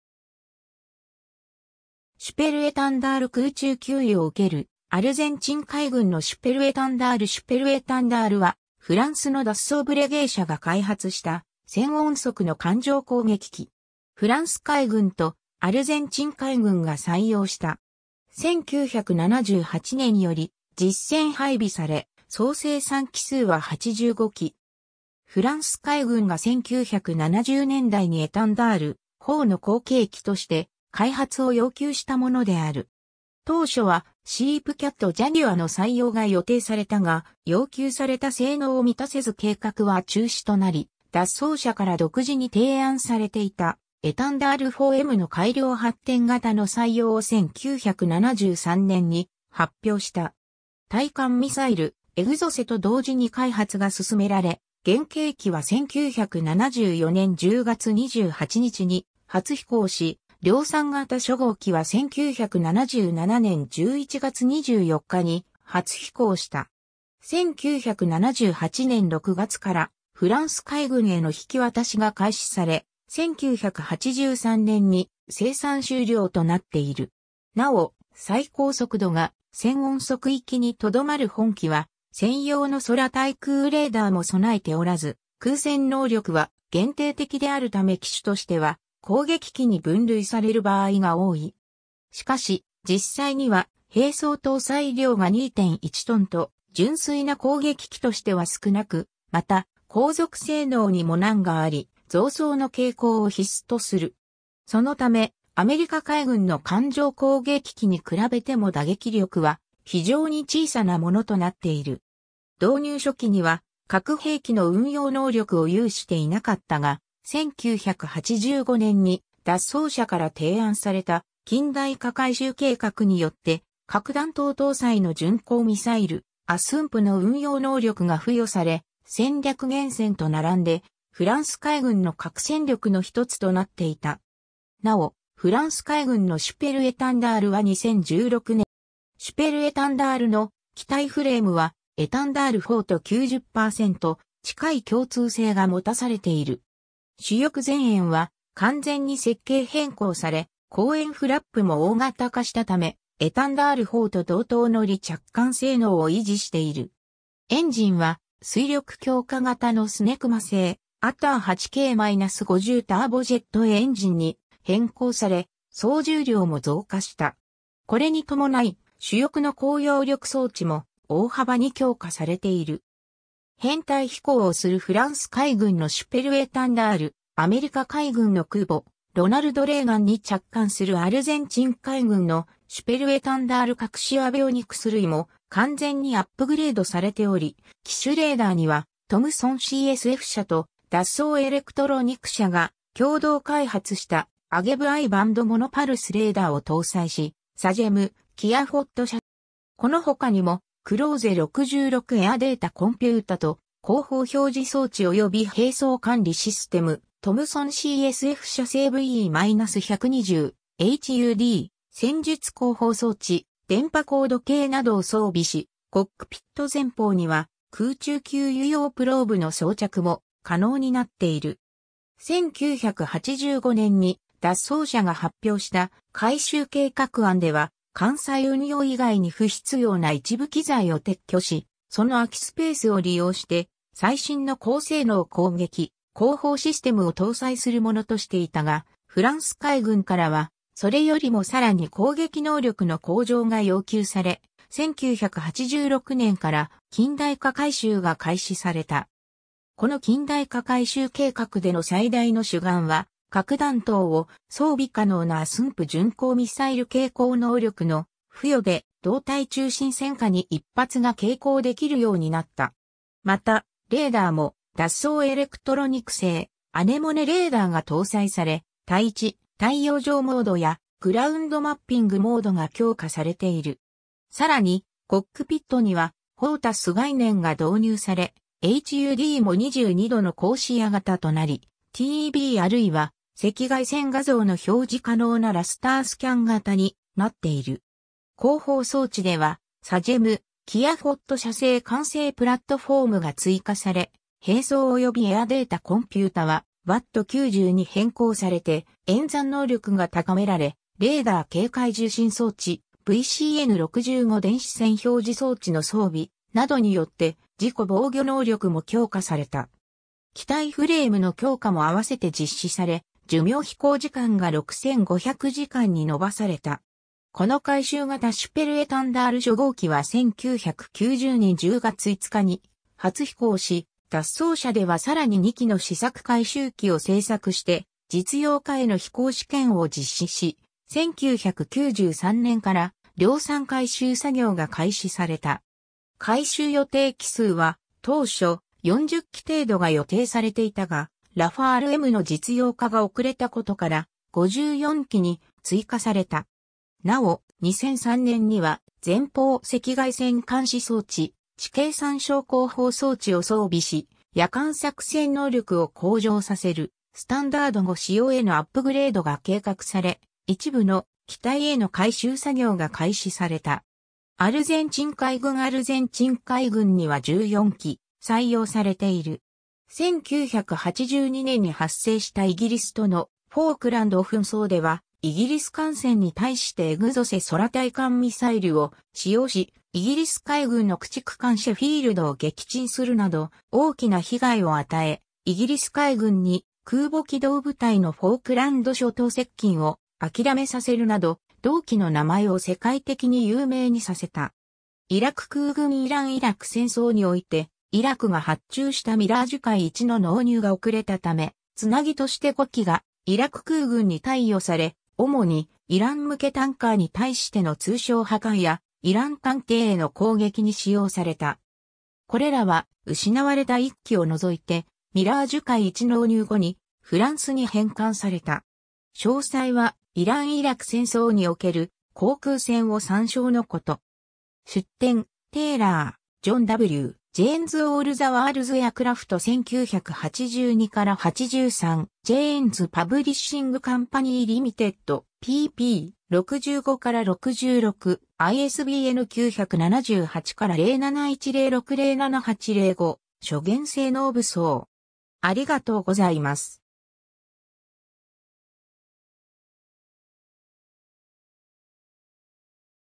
シュペルエタンダール
シュペルエタンダール 空中給油を受けるアルゼンチン海軍のシュペルエタンダール シュペルエタンダール（Super Étendard）は、フランスのダッソー・ブレゲー社が開発した遷音速の艦上攻撃機。
フランス海軍とアルゼンチン海軍が採用した。
1978年より実戦配備され、総生産機数は85機。
 フランス海軍が1970年代にエタンダールIVの後継機として開発を要求したものである。
当初はSEPECAT ジャギュアの採用が予定されたが、要求された性能を満たせず計画は中止となり、ダッソー社から独自に提案されていたエタンダールIV Mの改良発展型の採用を1973年に発表した。
対艦ミサイル「エグゾセ」と同時に開発が進められ、原型機は1974年10月28日に初飛行し、量産型初号機は1977年11月24日に初飛行した。
1978年6月からフランス海軍への引き渡しが開始され、1983年に生産終了となっている。
 なお、最高速度が遷音速域（M1.3程度まで）にとどまる本機は、専用の空対空レーダーも備えておらず、空戦能力は限定的であるため機種としては攻撃機に分類される場合が多い。
しかし、実際には兵装搭載量が2.1tと、純粋な攻撃機としては少なく、また、航続性能にも難があり、増槽の携行を必須とする。
そのため、アメリカ海軍の艦上攻撃機に比べても打撃力は非常に小さなものとなっている。
導入初期には核兵器の運用能力を有していなかったが、1985年にダッソー社から提案された近代化改修計画によって、核弾頭搭載の巡航ミサイル「ASMP」の運用能力が付与され、戦略原潜と並んでフランス海軍の核戦力の1つとなっていた。
 なお、フランス海軍のシュペルエタンダールは2016年7月13日退役、マルチロール機ラファールMへ更新された。
 シュペルエタンダールの機体フレームはエタンダールIVと90%近い共通性が持たされている。
主翼前縁は完全に設計変更され、後縁フラップも大型化したため、エタンダールIVと同等の離着艦性能を維持している。
エンジンは推力強化型のスネクマ製アター 8K-50 ターボジェットエンジンに変更され、総重量も増加した。
これに伴い、主翼の高揚力装置も大幅に強化されている。
 編隊飛行をするフランス海軍のシュペルエタンダール アメリカ海軍の空母「ロナルド・レーガン」に着艦するアルゼンチン海軍のシュペルエタンダール 各種アビオニクス類も完全にアップグレードされており、機首レーダーにはトムソンCSF社とダッソー・エレクトロニク社が共同開発したアゲブI バンド・モノパルス・レーダーを搭載し、サジェム/キアフォット社製ETNA航法/攻撃システム、サジェム/キアフォット社製SKN602慣性航法装置なども搭載されている。
このほかにも、クローゼ66 エア・データ・コンピュータと航法表示装置および兵装管理システム、トムソンCSF社製VE-120 HUD、戦術航法装置（TACAN）、電波高度計などを装備し、コックピット前方には空中給油用プローブの装着も可能になっている。
 1985年にダッソー社が発表した改修計画案では、艦載運用以外に不必要な一部器材を撤去し、その空きスペースを利用して最新の高性能攻撃/航法システムを搭載するものとしていたが、フランス海軍からは、それよりもさらに攻撃能力の向上が要求され、1986年から近代化改修が開始された。
この近代化改修計画での最大の主眼は、核弾頭を装備可能なASMP巡航ミサイル携行能力の付与で、胴体中心線下に1発が携行できるようになった。
また、レーダーもダッソー・エレクトロニク製アネモネ・レーダーが搭載され、対地/対洋上モードやグラウンド・マッピング・モードが強化されている。
さらに、コックピットにはHOTAS概念が導入され、HUDも22度の広視野型となり、TVあるいは赤外線画像の表示可能なラスター・スキャン型になっている。
 航法装置ではサジェム/キアフォット社製慣性プラットフォームが追加され、兵装およびエア・データ・コンピュータはUAT90に変更されて演算能力が高められ、レーダー警戒受信装置、VCN65電子戦表示装置の装備などによって自己防御能力も強化された。
機体フレームの強化も合わせて実施され、寿命飛行時間が6,500時間に延ばされた。
 この改修型シュペルエタンダール初号機は1990年10月5日に初飛行し、ダッソー社ではさらに2機の試作改修機を製作して実用化への飛行試験を実施し、1993年から量...